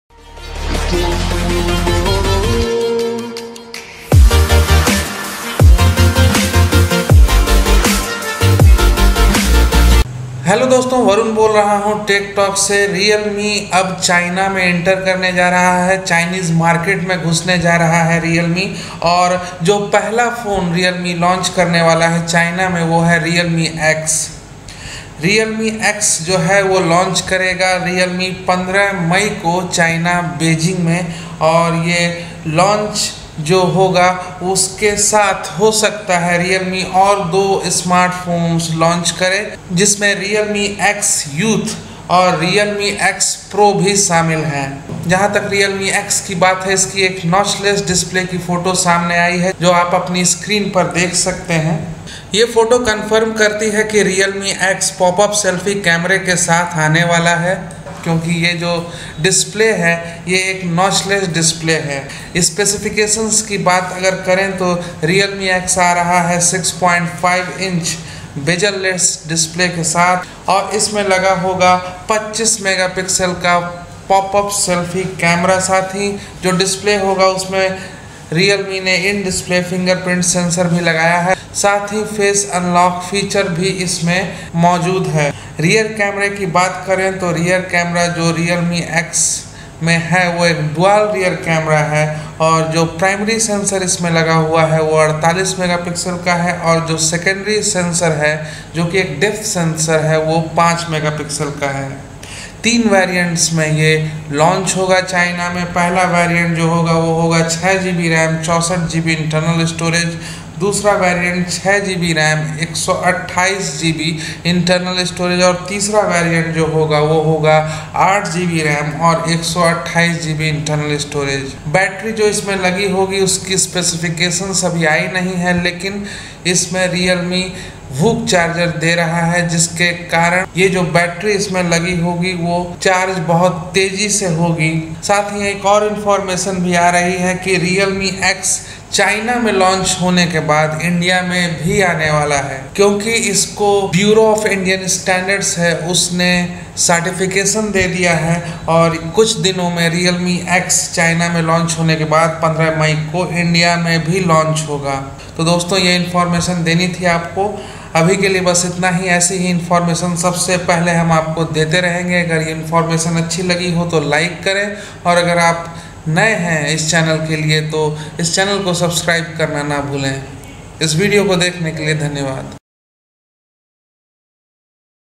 Realme X launch Date in China - India launch Soon
The Realme X is arriving in China on May 15, and just over a week ahead of its launch, a live image of the smartphone has been shared by the company's CMO Xu Qi Chase.

This photo shared by Chase shows off the phone's notchless display which is surrounded by slim bezels on its left, right, and top. The chin, however, is tad thicker. The notchless screen also re-confirms the presence of a pop-up selfie camera on the Realme X, making it the company's first smartphone to come with this feature.

As per a recent leak, the Realme X will be powered by the Snapdragon 730 SoC and will also feature an in-display fingerprint sensor. Support for VOOC 3.0 fast charging has also been rumoured, while the display will reportedly come with Corning Gorilla Glass 5 protection. In the imaging department, the Realme X will reportedly feature a 48-megapixel rear camera accompanied by a 5-megapixel depth camera.

The Realme X is tipped to arrive in three storage configurations, with the base 6GB RAM + 64GB storage variant carrying a price tag of CNY 1,599 (roughly Rs. 16,500). The 6GB RAM + 128GB storage variant and the top-end 8GB RAM + 128GB storage version of Realme X will be priced at CNY 1,799 (roughly Rs. 18,500) and CNY 1,999 (roughly Rs. 20,600) respectively.